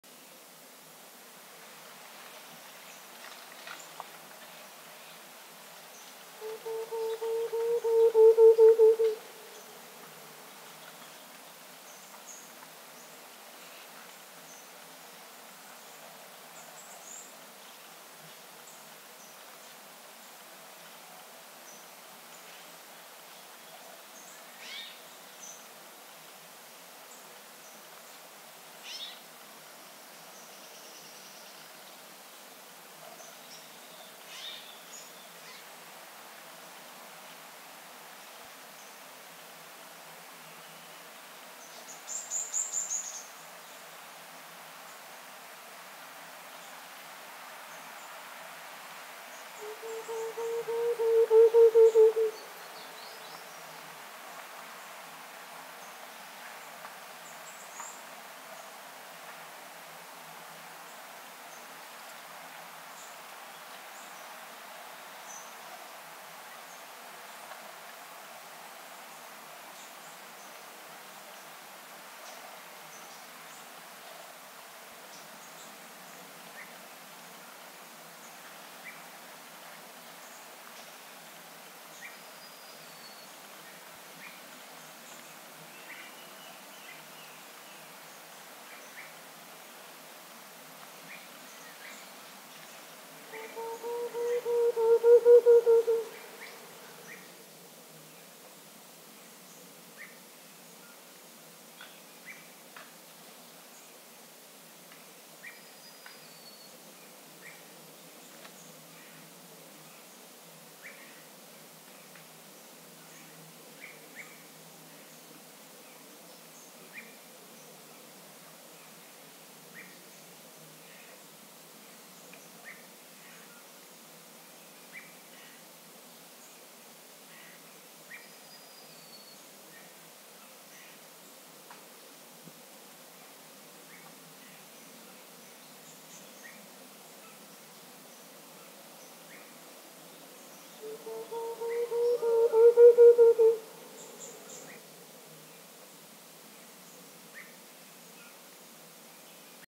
Tovacuçu / Variegated Antpitta
A vocalizaão do indíviduo, realizada após a filmagem, e que ocorre em média a cada 40'', foi mixada no vídeo sem nenhuma relação ou sincronismo com o vídeo.